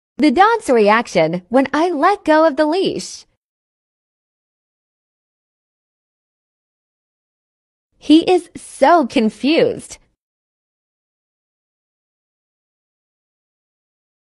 The dogs reaction when I let go of leash🐾❣️rottweiler
it's the Rottweiler world
Rottweiler Rotties videos,
Rottweiler compilation videos, learn more about Rottweiler , the beautiful creature Rottweiler,